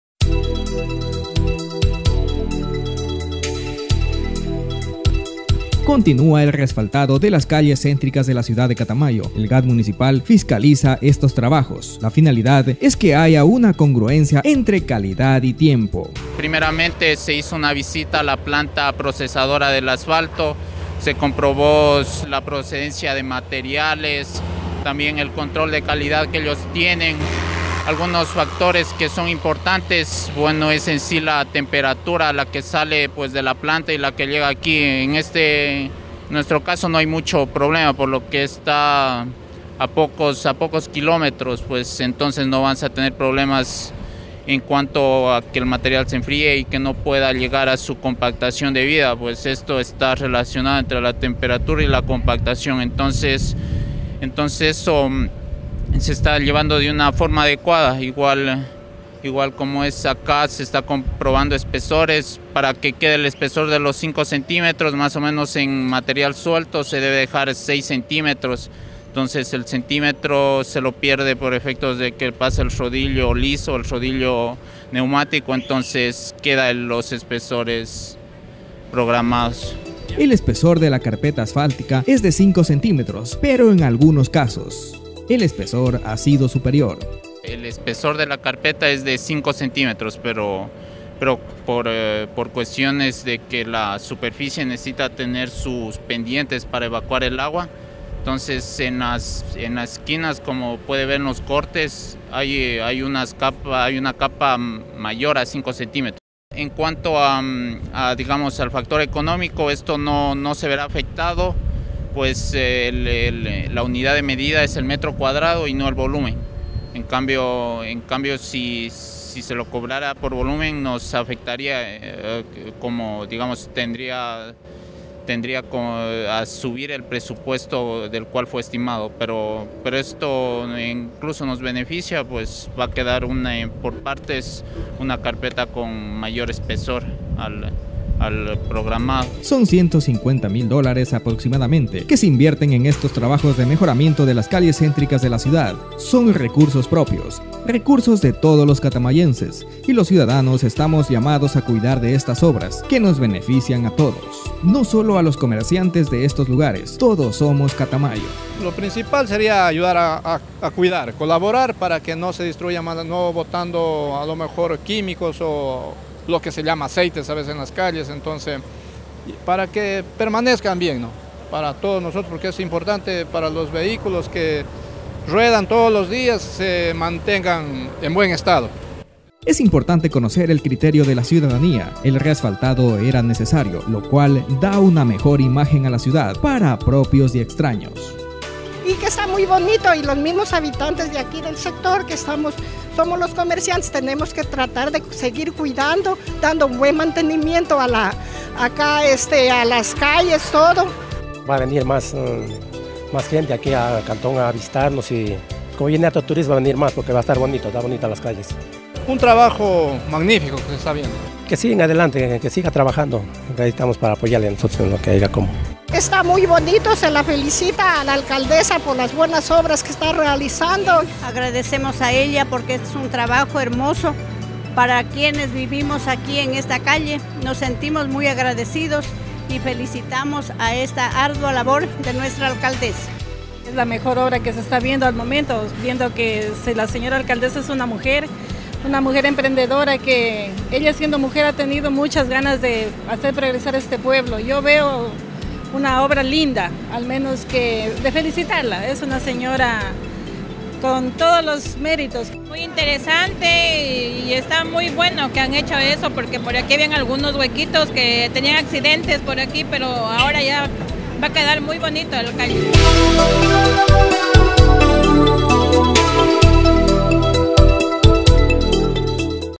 Catamayo.- GADMCatamayo Fiscaliza el recapeo de calles centrícas
Continúan el reasfaltados de las calles céntricas de la ciudad de Catamayo, el GAD Municipal  fiscaliza estos los trabajos, la finalidad es que haya una congruencia entre tiempo y calidad del cumplimiento de estas obras, por parte de la empresa contratada.
El espesor de la capa asfáltica es de 5cm., pero en algunos ha sido superior.

150.000 USD aprox., se invierten en estos trabajos de mejoramiento de calles céntricas de Catamayo, son recursos propios, recursos de todos los Catamayenses, y los ciudadanos estamos llamados a cuidar de estas obras que nos benefician a todos, no solo a los comerciantes de estos lugares, todos somos Catamayo.

Es importante, conocer el criterio de la ciudanía, el resfaltado era necesario lo cual da una mejor imagen a la ciudad, para propios y extraños.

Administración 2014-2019